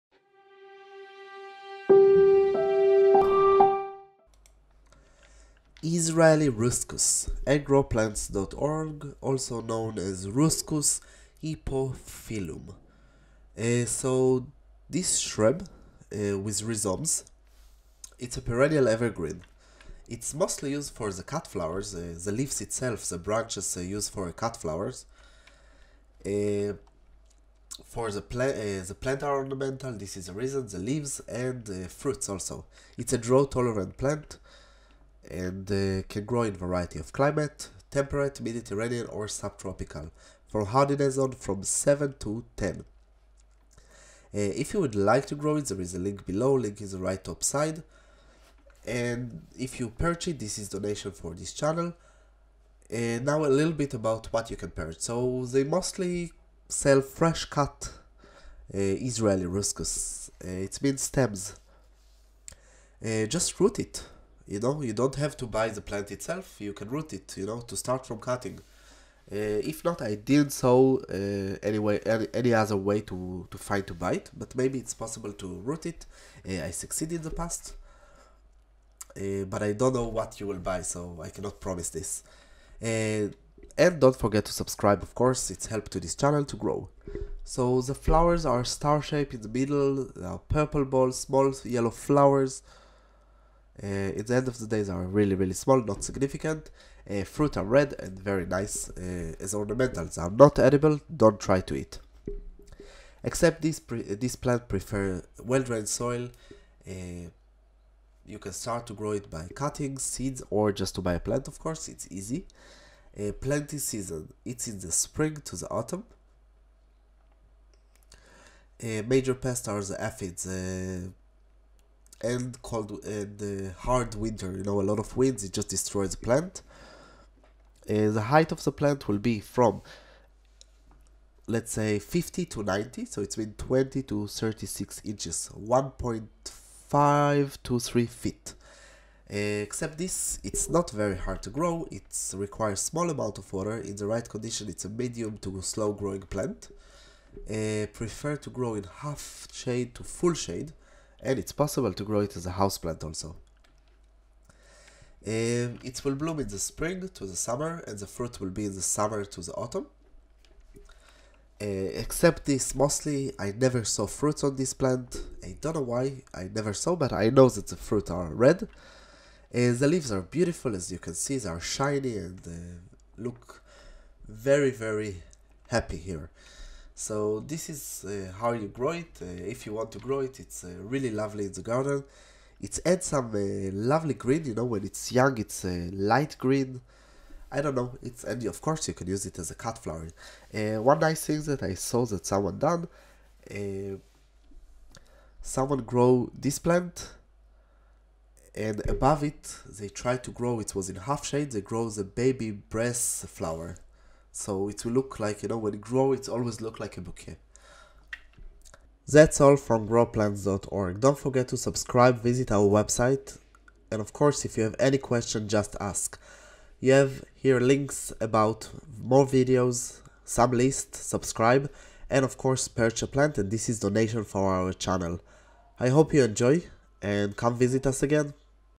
Israeli Ruscus - grow & care (Butcher’s-broom)
Israeli Ruscus for sale - All what need to know before buy the plant.